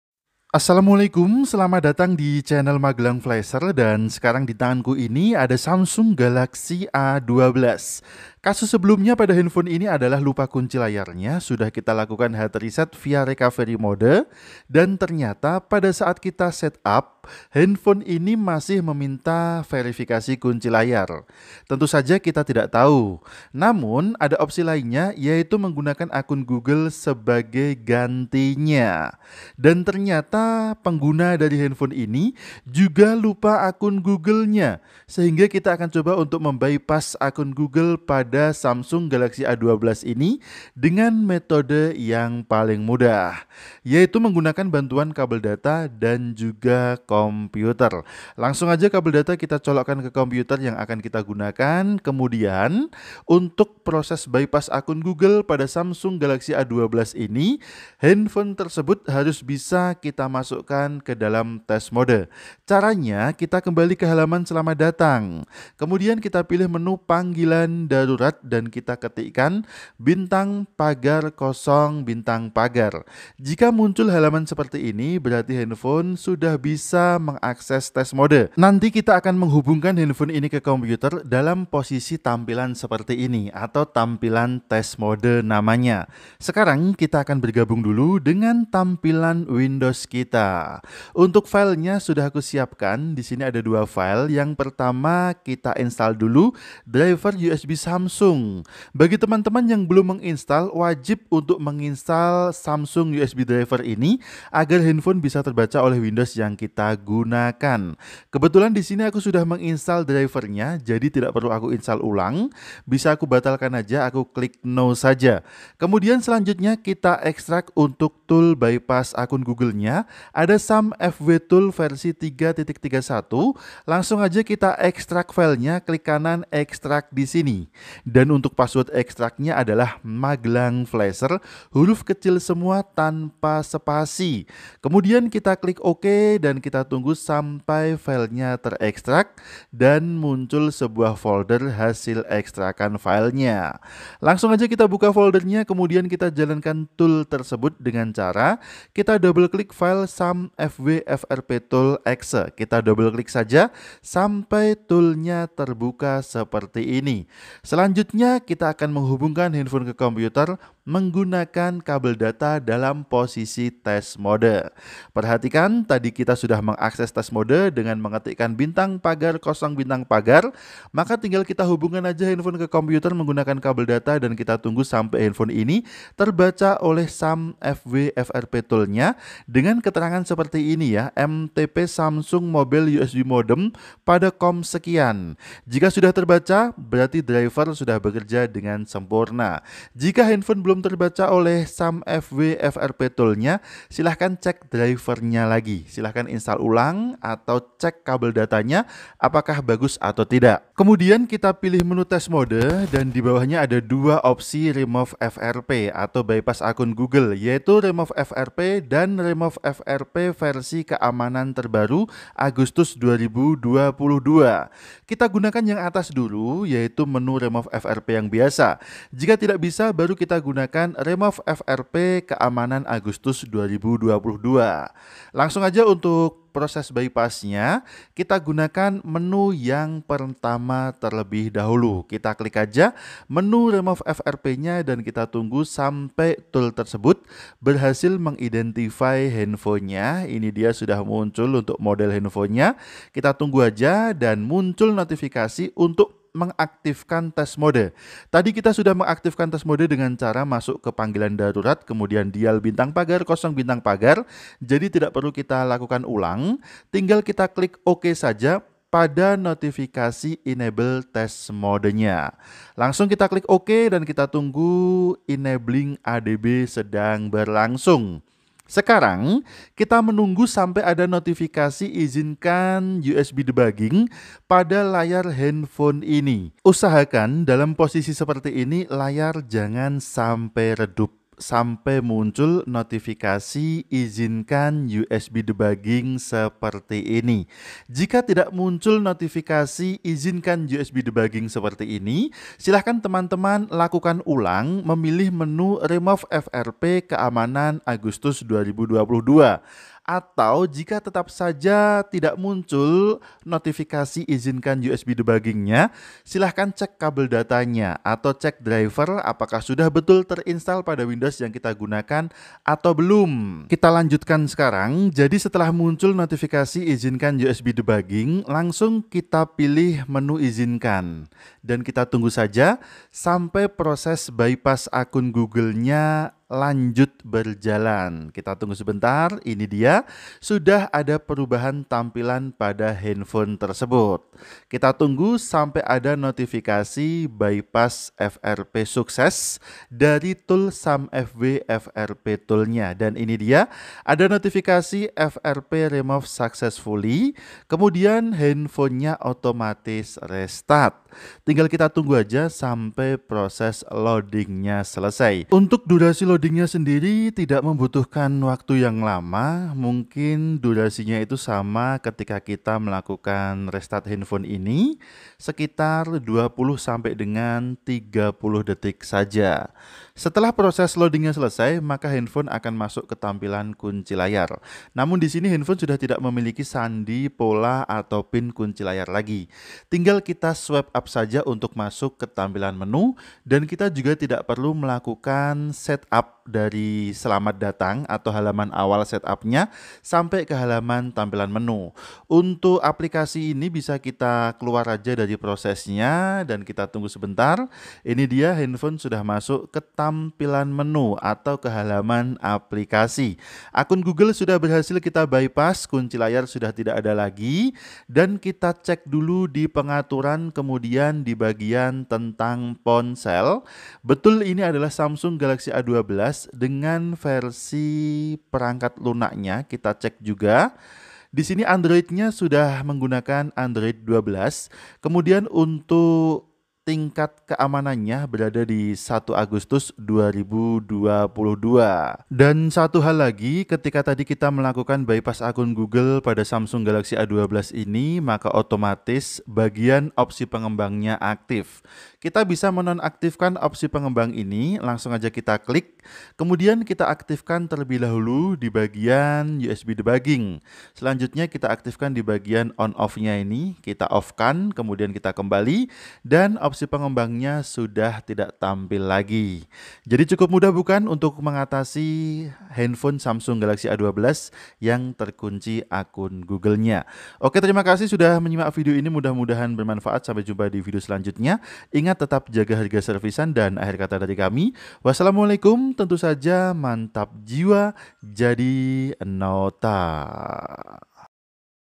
Cara Bypass Frp Samsung Galaxy A12 Lupa Akun Google Gratis Josssss
Cara Bypass Frp Samsung Galaxy A12 Lupa Akun Google dengan tool gratis dan cukup sekali klik saja. Untuk tool nya bisa diunduh pada tautan dibawah ini.